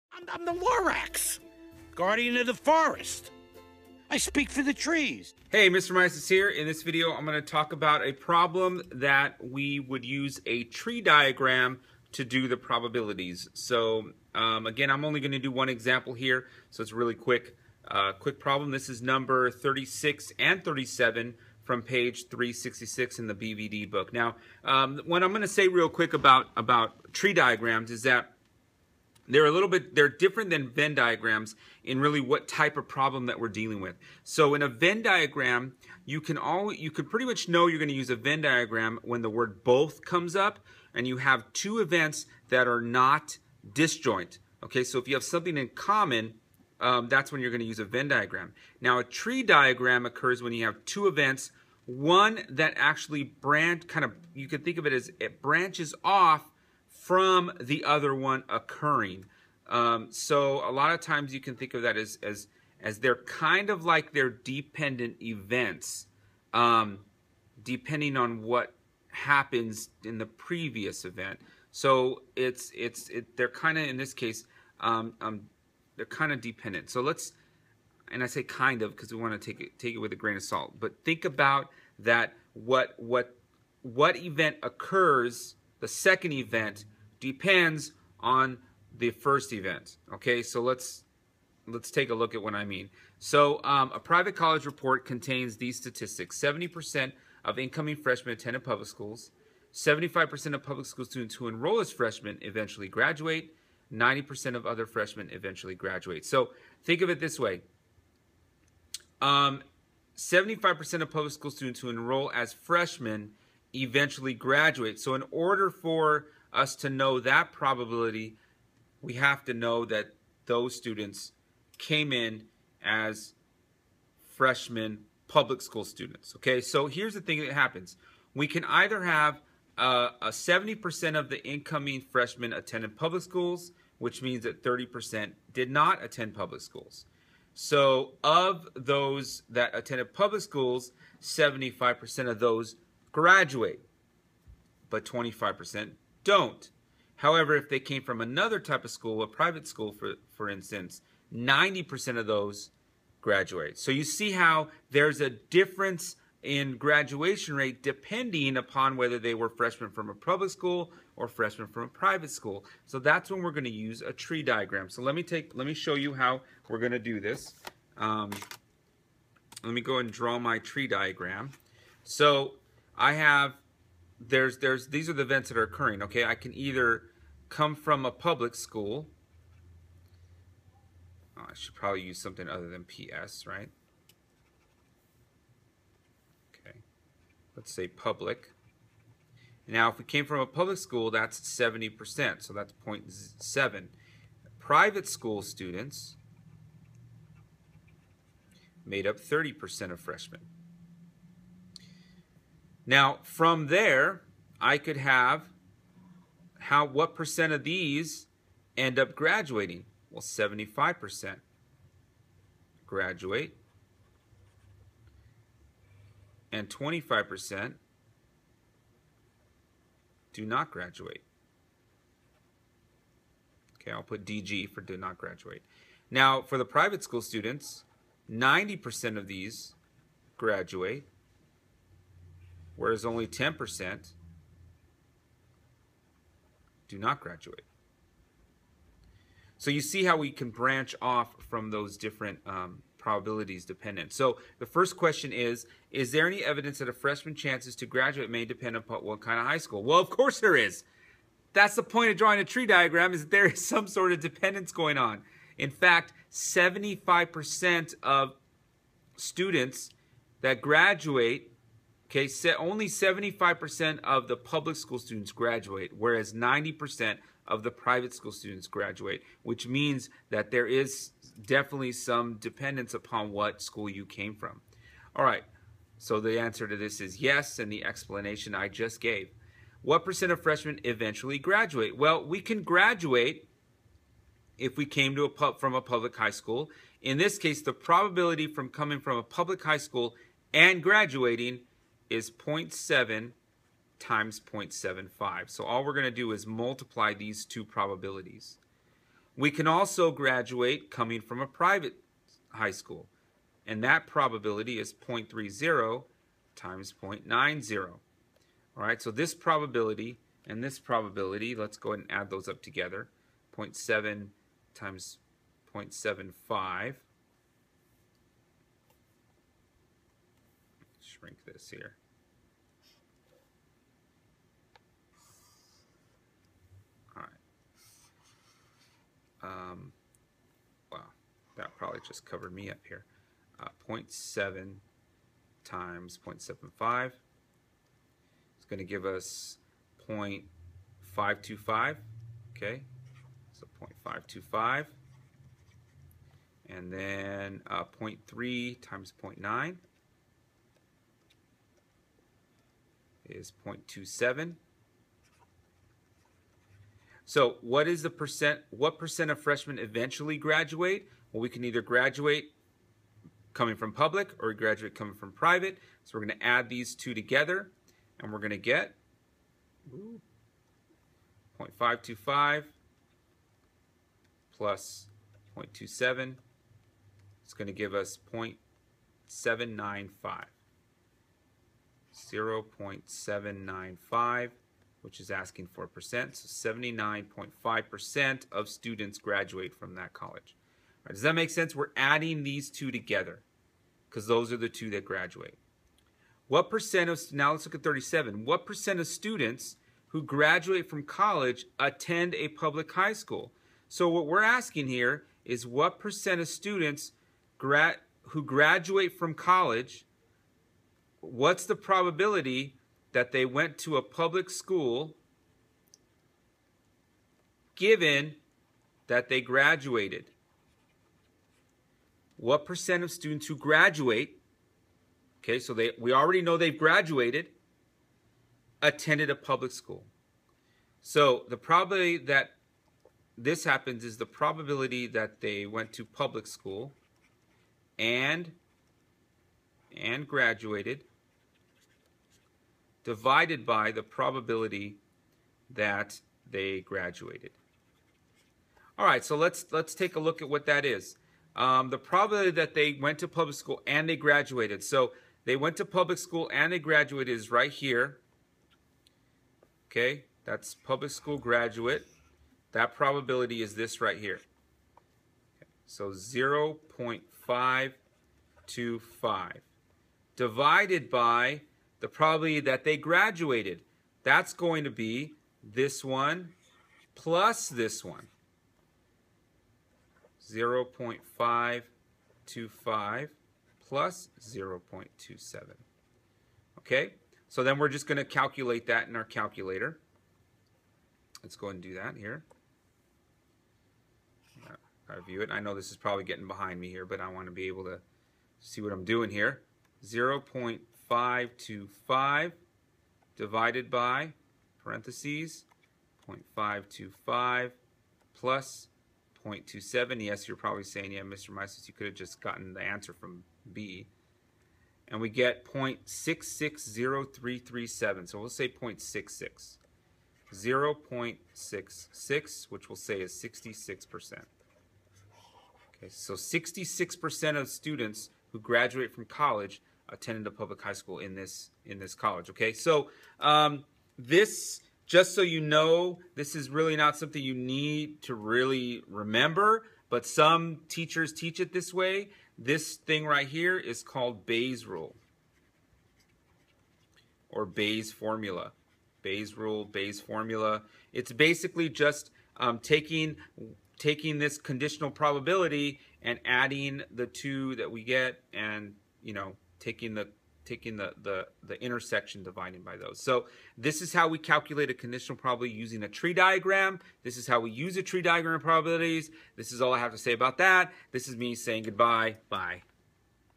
Probabilities using Tree Diagrams
In this video, I discuss how to use tree diagrams to compute probabilities and using Baye's Formula. AP Stats WSQ